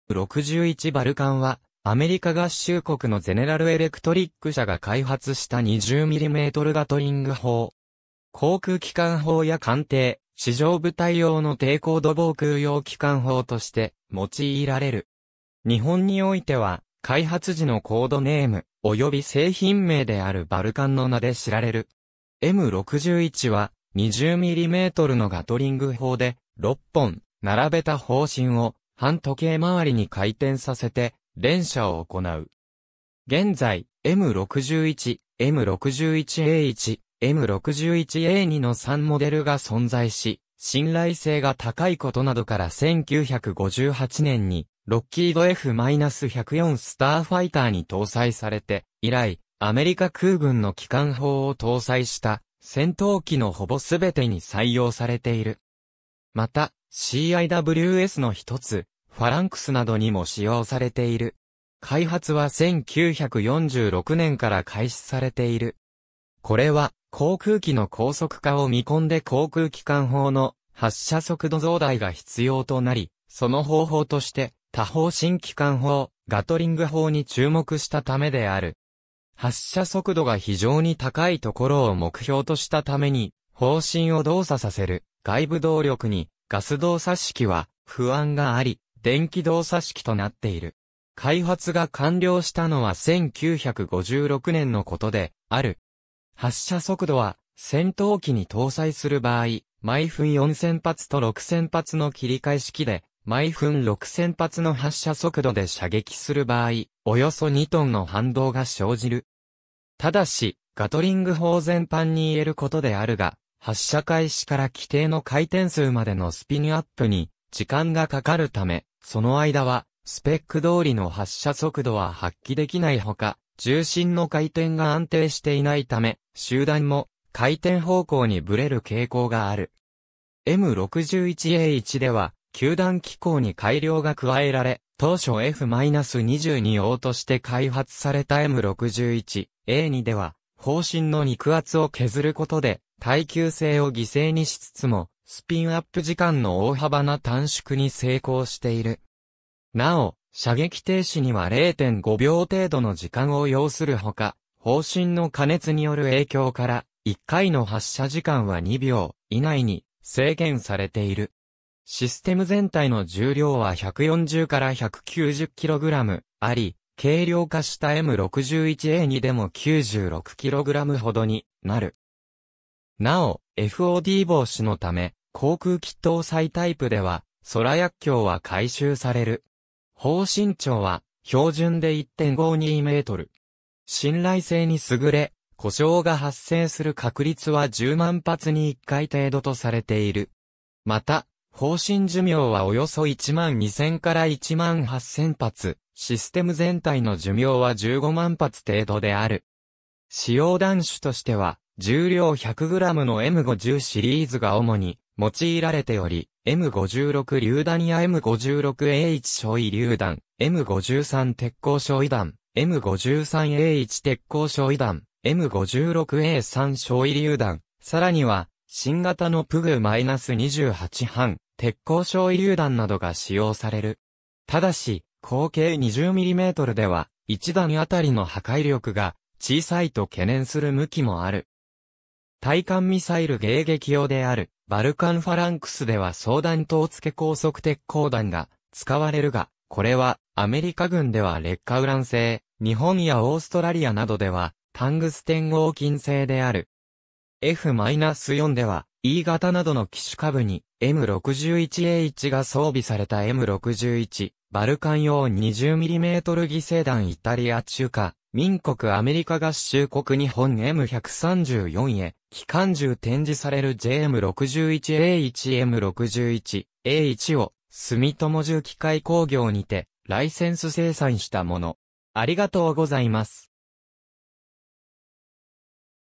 M61 バルカン
ゼネラル・エレクトリック
M61 バルカン (M61 Vulcan) は、アメリカ合衆国のゼネラル・エレクトリック (GE) 社が開発した20mmガトリング砲。
航空機関砲や艦艇・地上部隊用の低高度防空用機関砲として用いられる。
日本においては、開発時のコードネーム、および製品名であるバルカン（Vulcan：ローマ神話に登場する火神。ギリシア神話の鍛冶神ヘーパイストスに相当する。） の名で知られる。
M61は、20mmのガトリング砲で、6本並べた砲身を反時計周りに回転させて連射を行う。
現在、M61、M61A1、M61A2 の3モデルが存在し、信頼性が高い事などから1958年にロッキードF-104 スターファイターに搭載されて以来、アメリカ空軍の機関砲を搭載した戦闘機のほぼすべてに採用されている。
また、CIWS（近接防御火器システム）の一つ、ファランクスなどにも使用されている。
開発は1946年から開始されている。
これは、航空機の高速化を見込んで航空機関砲の発射速度増大が必要となり、その方法として多砲身機関砲・ガトリング砲に注目したためである。
発射速度が非常に高いところを目標としたために、砲身を動作させる外部動力にガス動作式は不安があり、電気動作式となっている。
開発が完了したのは1956年のことである。
発射速度は戦闘機に搭載する場合、毎分4,000発と6,000発の切り替え式（最大で毎分7,200発、試験的に行った記録では12,000発）で、毎分6,000発の発射速度で射撃する場合、およそ2tの反動が生じる。
ただし、ガトリング砲全般にいえることであるが、発射開始から規定の回転数までのスピンアップに時間がかかる（風車発電式ガンポッドで数秒、電動式で1秒弱、油圧式で0.3秒程度）ため、その間はスペック通りの発射速度は発揮できない（油圧式の場合、射撃開始後1秒間の発射速度は毎秒70発程度）ほか、銃身の回転が安定していないため、集弾も回転方向にぶれる傾向がある。
M61A1では給弾機構に改良が加えられ、当初F-22用として開発されたM61A2では砲身の肉厚を削ることで耐久性を犠牲にしつつもスピンアップ時間の大幅な短縮に成功している。
なお、射撃停止には0.5秒程度の時間を要する（この間、砲弾は発射されないが、供給される砲弾が未使用のまま弾倉へ収容される）ほか、砲身の加熱による影響から、1回の発射時間は2秒以内（おおむね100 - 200発相当）に制限されている。
システム全体の重量は140-190kgあり、軽量化したM61A2でも96kgほどになる。
なお、FOD（エンジンの異物吸入による損傷）防止のため、航空機搭載タイプでは空薬莢は回収される。
砲身長は標準で1.52m。
信頼性に優れ、故障が発生する確率は100,000発に1回程度とされている。
また、砲身寿命はおよそ12,000 - 18,000発、システム全体の寿命は150,000発程度である。
使用弾種としては、重量100グラムのM50 シリーズが主に用いられており、M56榴弾やM56A1焼夷榴弾、M53徹甲焼夷弾、M53A1徹甲焼夷弾、M56A3焼夷榴弾、さらには新型のPGU-28半徹甲焼夷榴弾などが使用される。
ただし、口径20mmでは、1弾あたりの破壊力が小さいと懸念する向きもある。
対艦ミサイル迎撃用であるバルカン・ファランクスでは装弾筒付高速徹甲弾 (APDS) が使われるが、これはアメリカ軍では劣化ウラン製、日本やオーストラリアなどではタングステン合金製である（アメリカ海軍でも1988年以降はタングステン合金製弾芯を使用しているとされている）。
F-4では、E型などの機首下部にM61A1が装備された（写真はF-4EJ） M61 バルカン用20mm擬製弾（JM51A1） イタリア 中華民国 アメリカ合衆国 日本 M134重機関銃 展示されるJM61A1 M61A1を住友重機械工業にてライセンス生産したもの